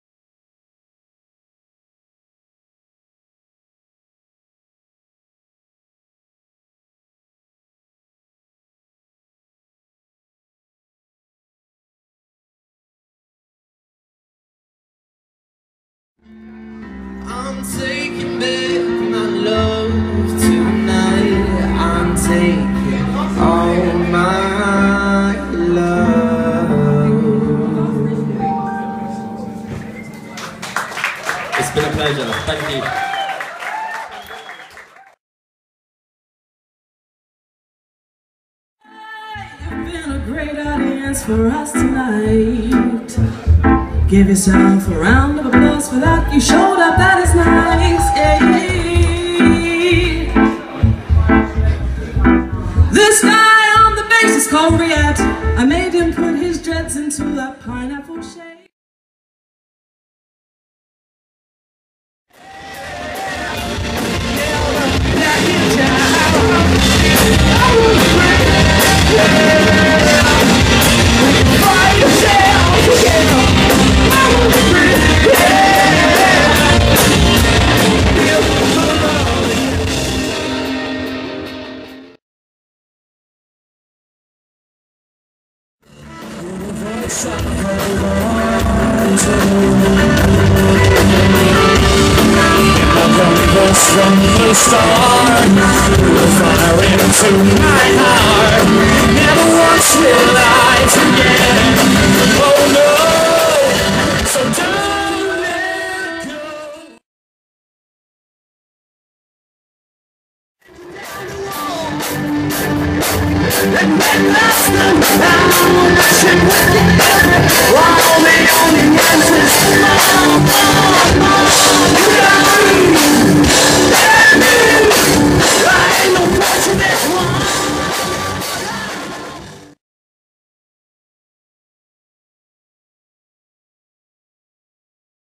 Discovery 2 - 30th April 2015
Discovery 2 at 229 The Venue, Great Portland Street, London. Video filmed/made by 140musicpromo .